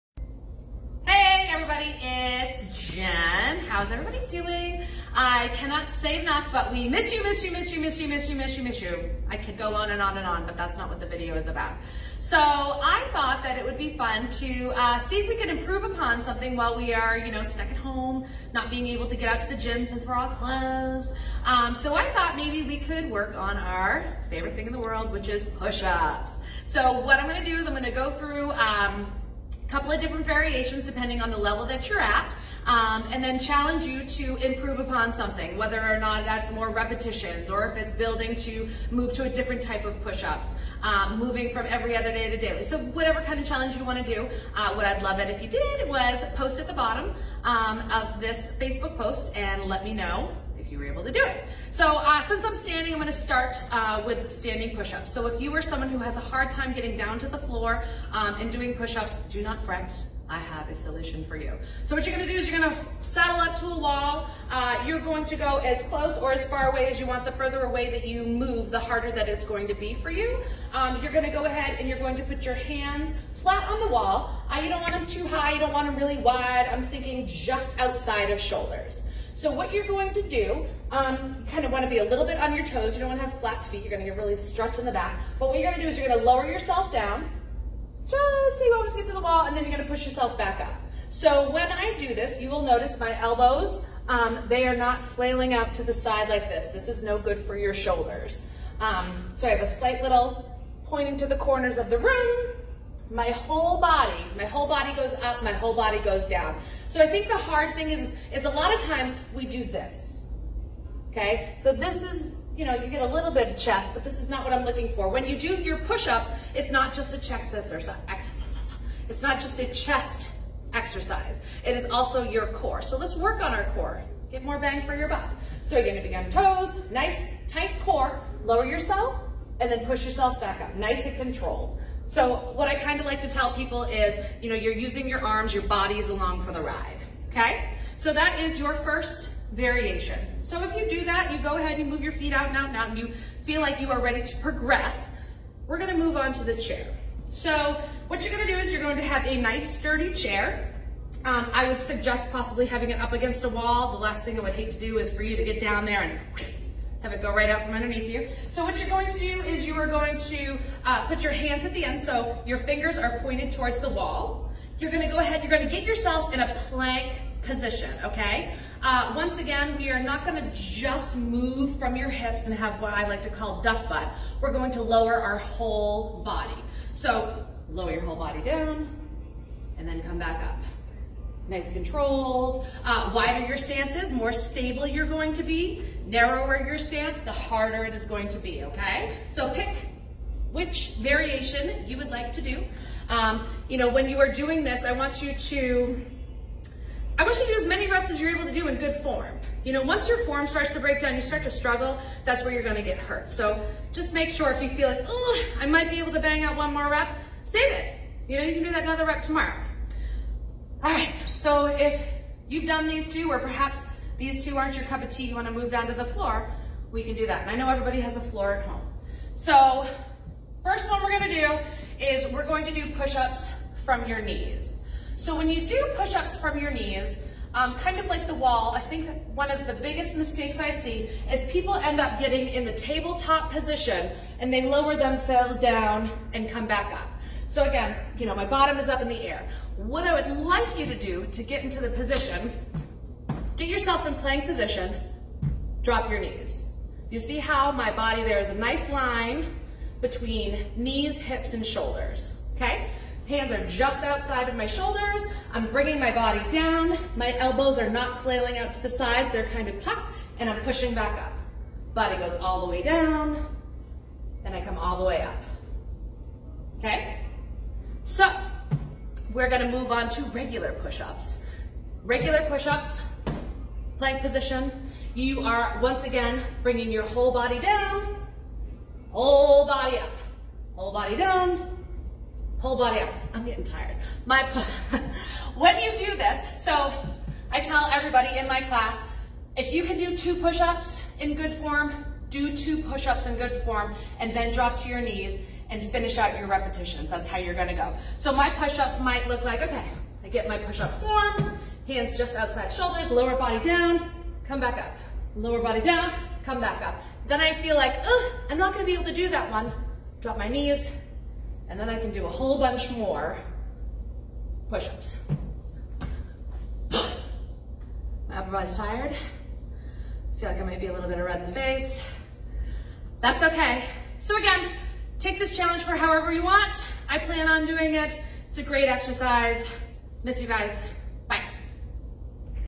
Push Ups - YMCA of Strafford County
Jen, from the YMCA of Strafford County, is here to help you work on your push ups while you're at home!